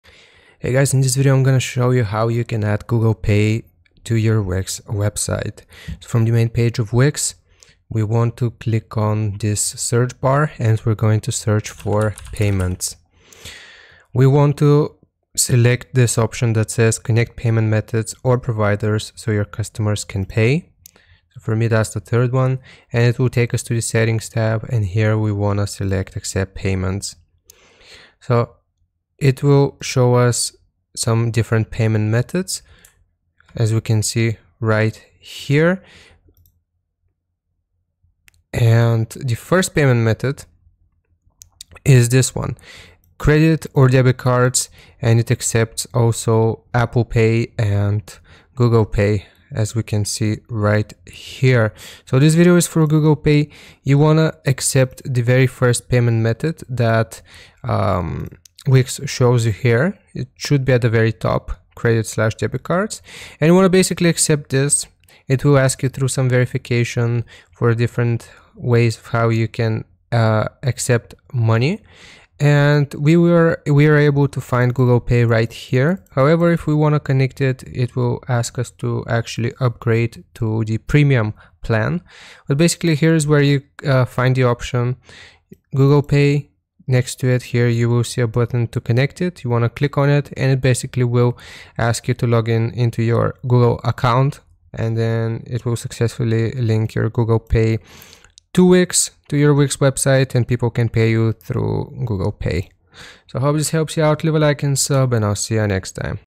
How To Add Google Pay To Wix Website
In this video I show you how to add Google Pay to Wix website! If you want to learn how to accept payments with Google Pay on your website created by Wix then this is the tutorial for you.